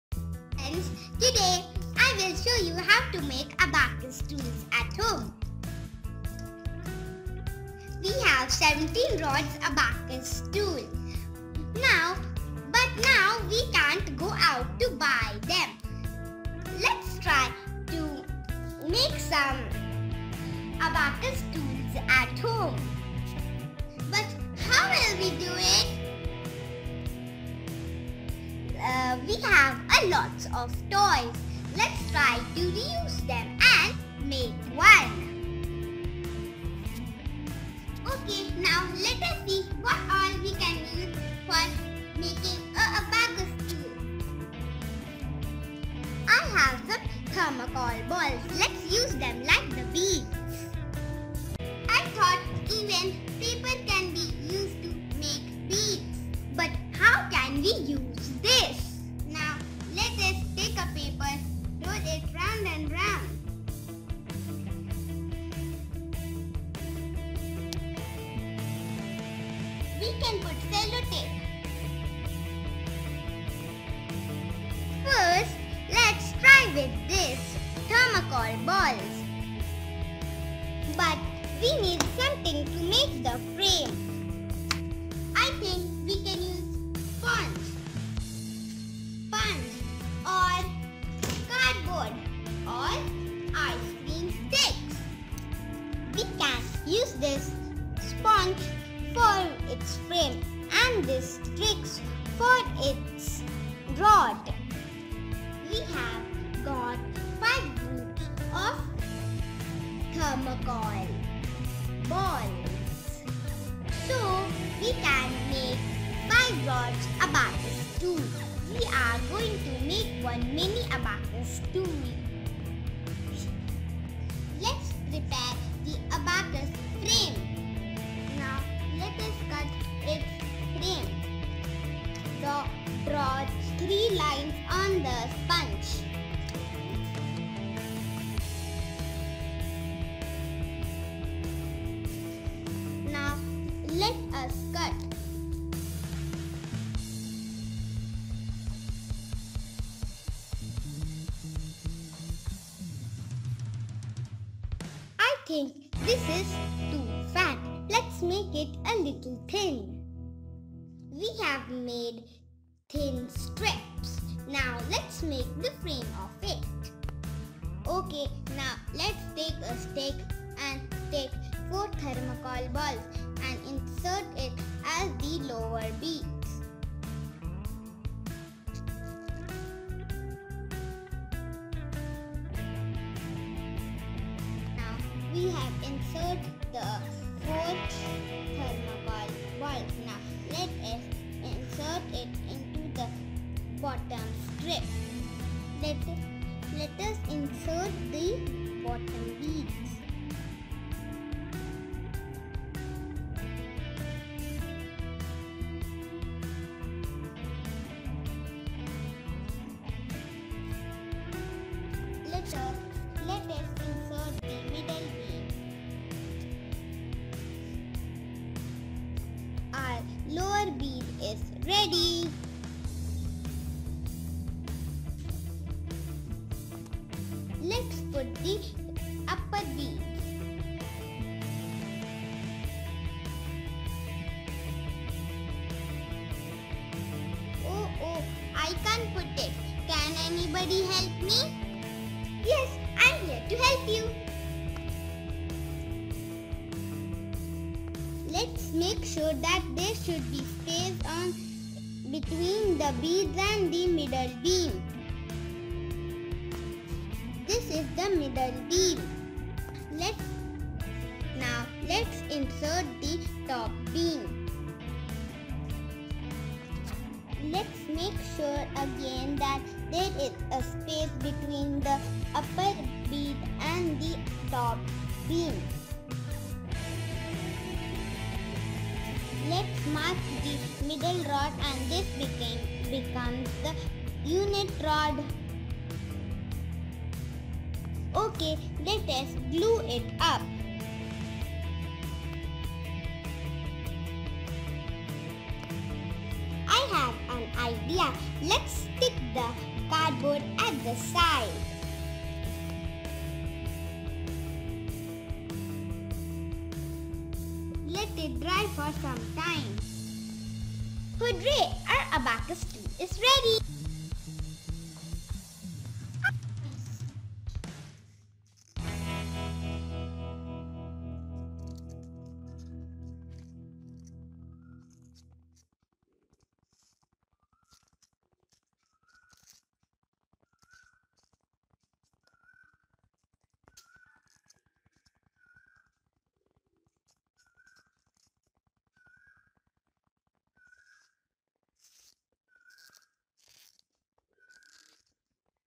Kids Abacus Tool - How to make abacus tool at home? - DIY Easy to Make Mini Abacus Tool for children
Let us learn how to make Kids miniAbacus Tool in less than an hour. Do try yourself and share your experience.

We recommend to follow these eAbacus Practices regularly to get the best results.

Happy Learning